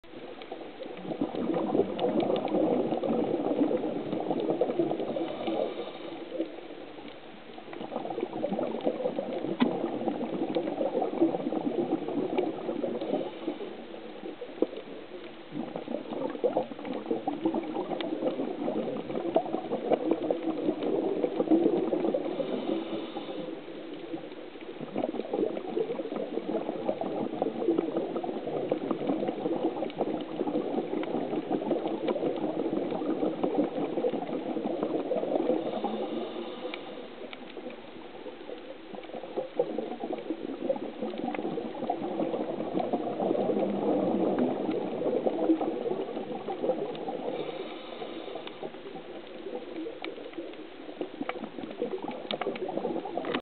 Triggerfish battle
I have not seen this one before. kinda wild since they would sort of check to see if I was still there. Felt like I was suppose to cheer or congratulate the winner.